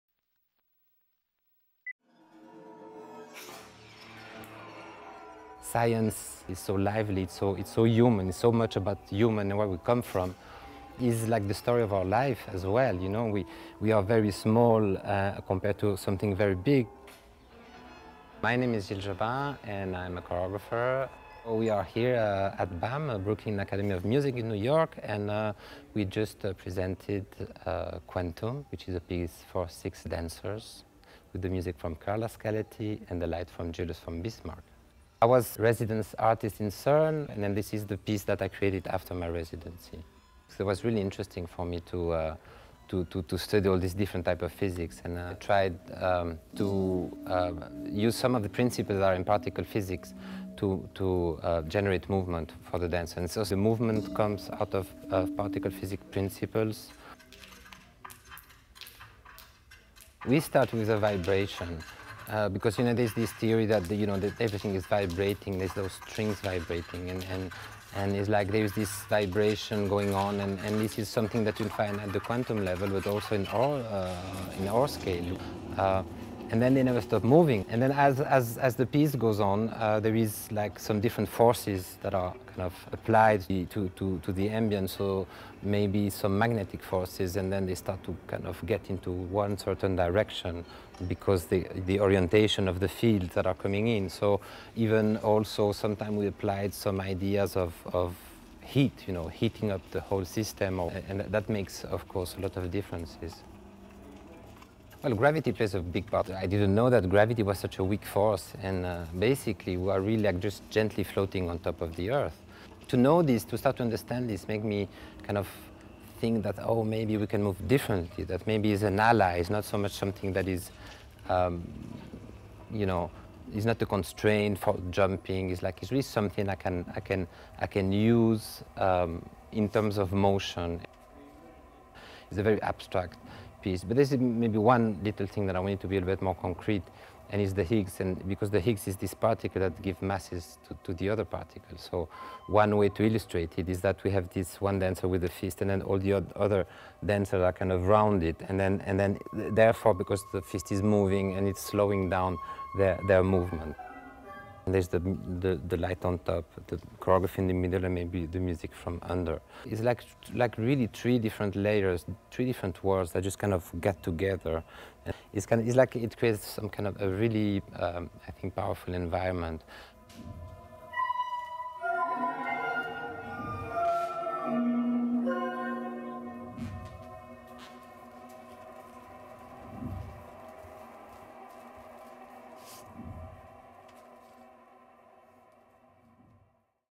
Quantum: An Ode to Particle Physics
Drawing on his experience as Artist-in-Residence at CERN, Gilles Jobin's dance performance, Quantum, presents an abstract meditation on the motion of particles and laws of physics.